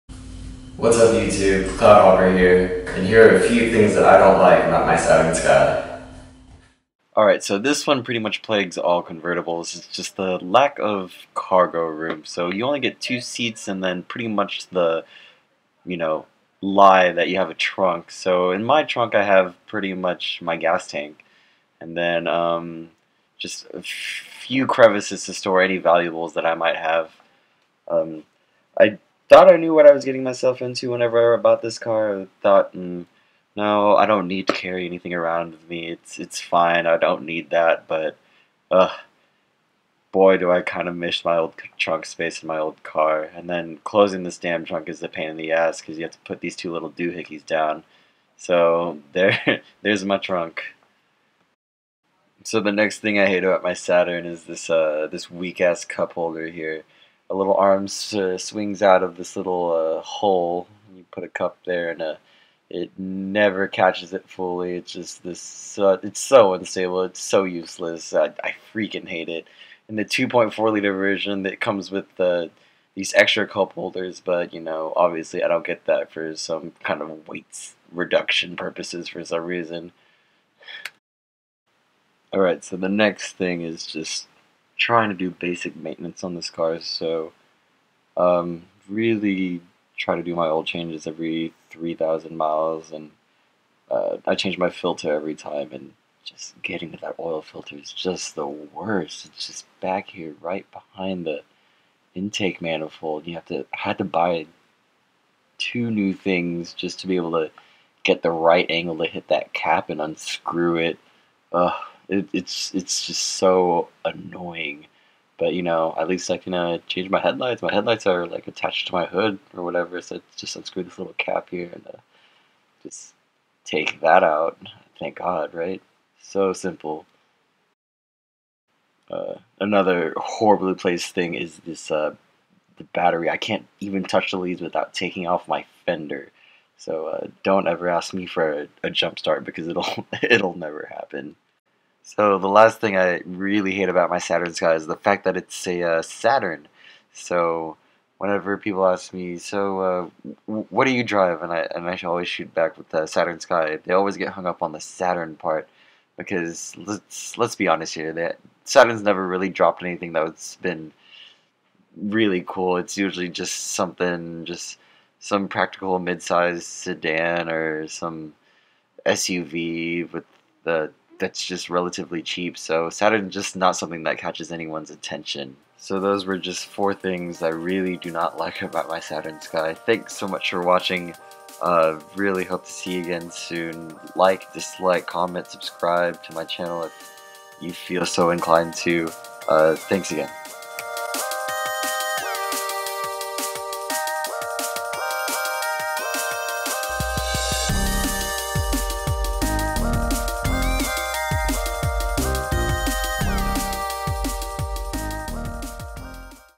4 Things I Hate About My Saturn Sky
4 Things I Hate About My Saturn Sky Redline

-Cargo Room
-Cup Holder
-Maintenance
-Saturn